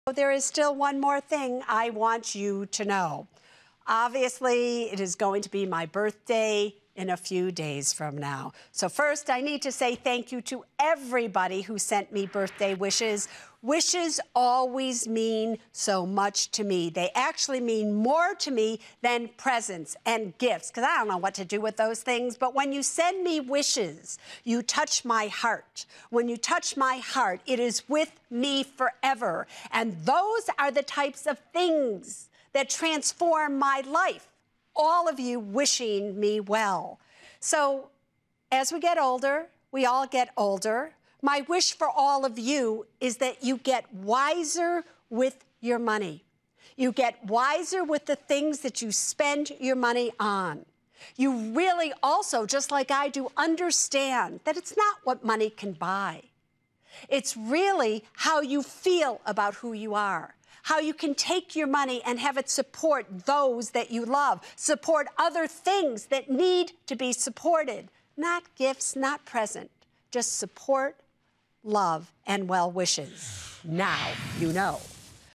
Suze Orman on birthdays
Suze Orman explains the value of giving birthday wishes instead of presents.